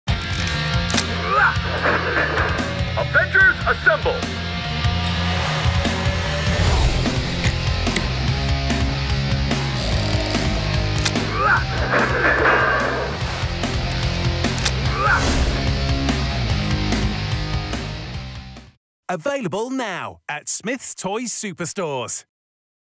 Marvel: Avengers Captain America Figure 33cm- Smyths Toys
Fans and collectors can imagine the action-packed scenes of the Marvel universe with this premium Captain America figure.